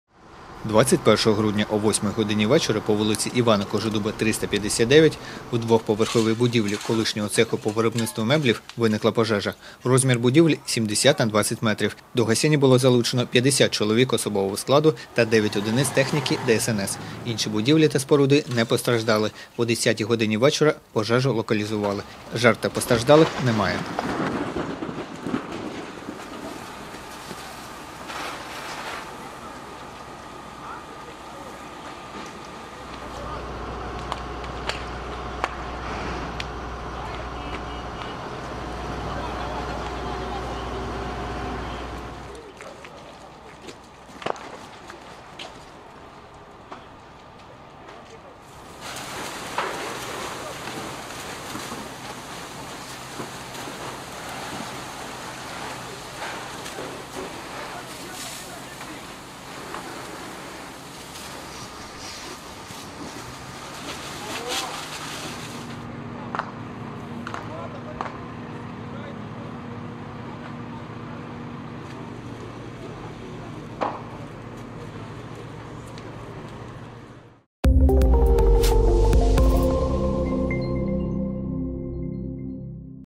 Пожежа цеху по виробництву меблів у Білій Церкві 🔥
21 грудня о 20:24 по вул. Івана Кожедуба, 359 в двоповерховій будівлі колишнього цеху по виробництву меблів виникла пожежа
Розмір будівлі 70х20 м
До гасіння було залучено 50 чол. о/с та 9 од. техніки ДСНС.
інші будівлі та споруди не постраждали
О 21:57 пожежу локалізували.
Жертв та постраждалих немає.